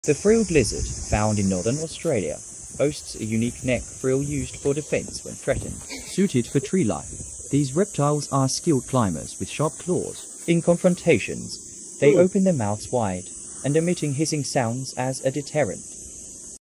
Frill Neck Lizard Attacks!
The frilled lizard, native to northern Australia and southern New Guinea, is recognized for its distinctive neck frill. This unique feature is displayed when the lizard feels threatened, creating an intimidating spectacle. Adapted to arboreal life, these reptiles have slender bodies, long tails, and sharp claws for climbing. They primarily feed on insects and small vertebrates, using their agility and keen eyesight. When confronted, frilled lizards open their mouths wide, displaying a colorful interior, and emit hissing sounds as a deterrent. Despite their intimidating displays, they are generally shy and prefer retreating to the treetops. The frilled lizard's captivating behaviors have made it a notable species in documentaries and wildlife programs, earning admiration from reptile enthusiasts and nature lovers.